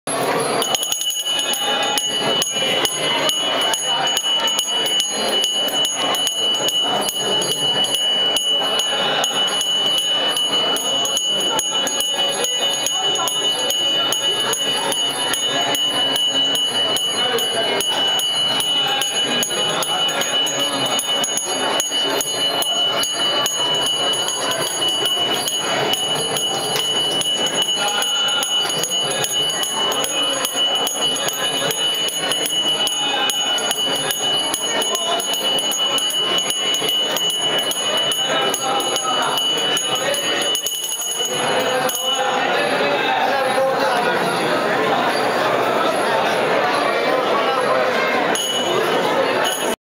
Madurai Shiva worship with candles
At the big temple in Madurai, Carrie and  stumbled upon this nightly puja. Inside the chariot is supposedly Shiva, who is being brought to bed by prayer.